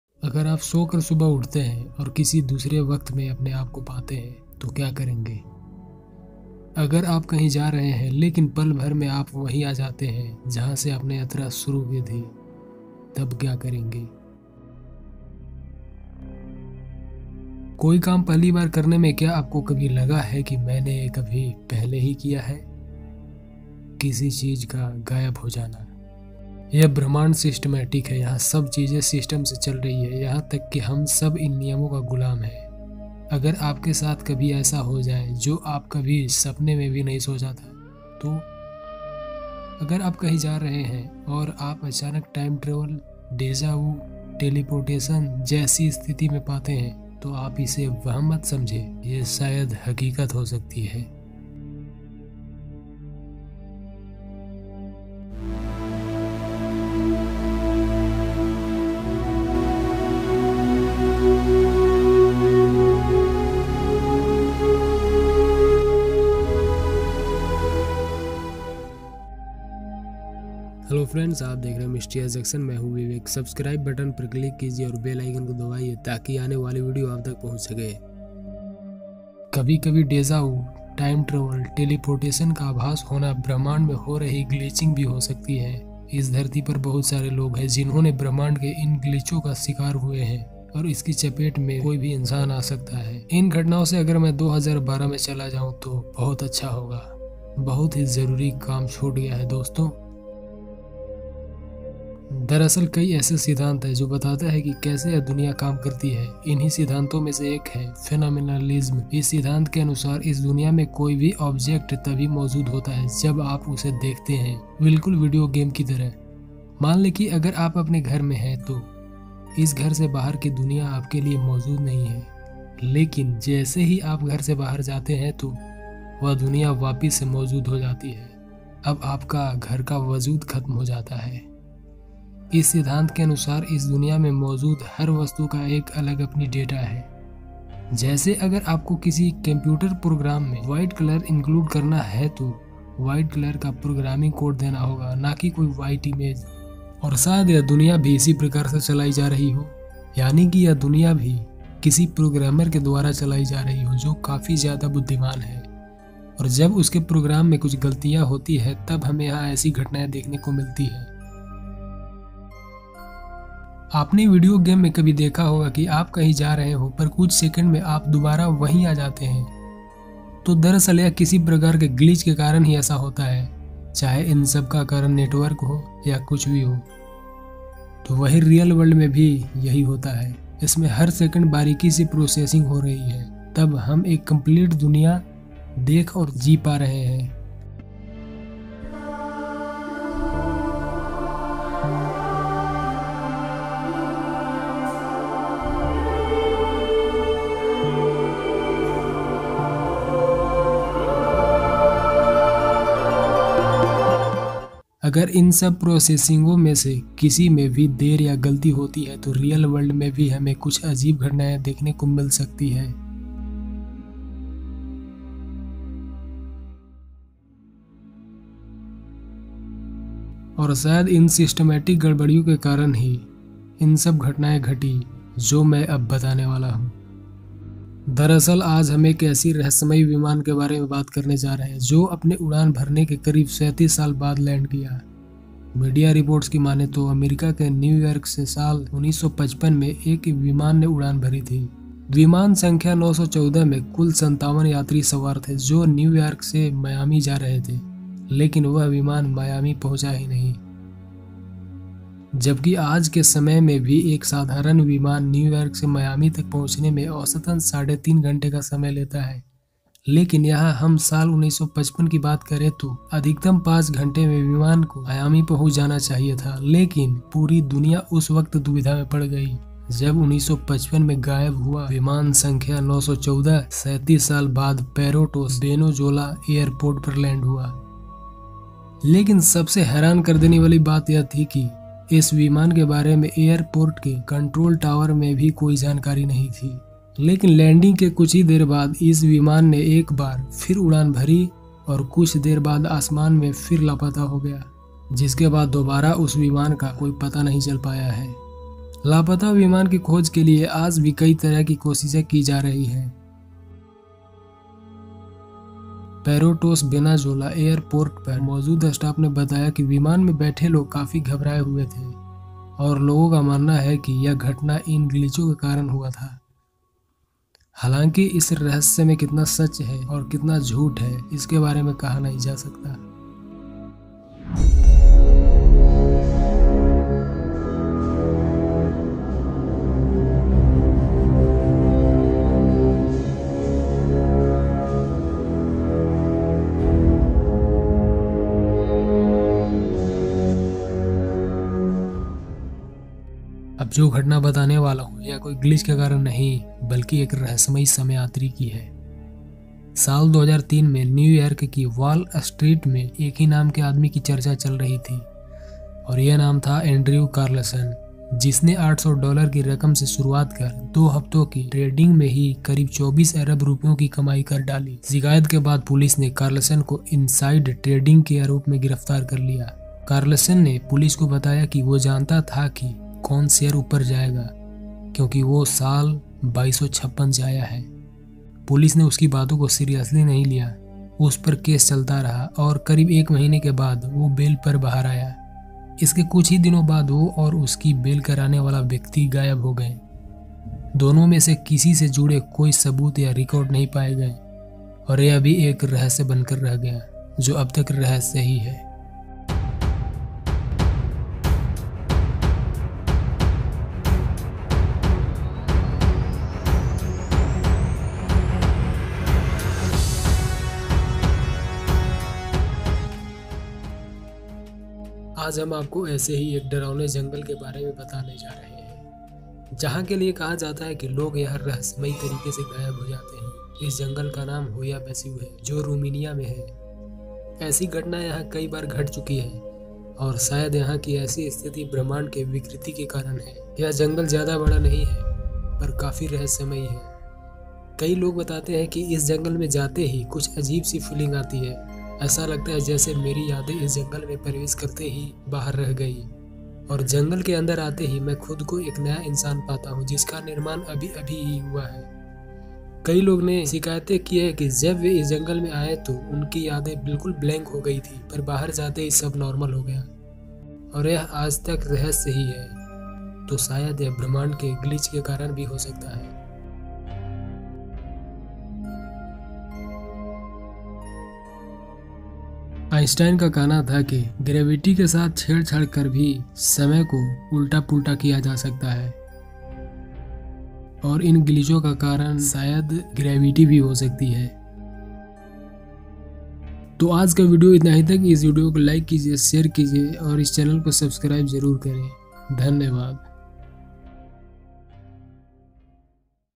समय यात्रा की घटनाएं क्यों होती हैं?we can time travel by universe glitches
What will you do if you wake up in the morning and find yourself at some other time. If you are going somewhere, but in a moment you come to where you started your journey, what to do?  Do you sometimes think I have ever done this? To make something disappear…

This universe is systematic, here everything is going on with the system, even we are all slaves to these rules.

If something happens to you that you never thought of in your dream?  If you are going somewhere and you suddenly find yourself in a situation like time travel, deza u, teleportation, then you should not consider it.

Sometimes the feeling of deja au, time travel, teleportation can also be the glitches happening in the universe. There are many people on this earth who have fallen victim to these glitches of the universe.  And any human can come in its grip.

If I go away from these events in 2012, it would be very good. Very important work friends.

some information source respected news channel.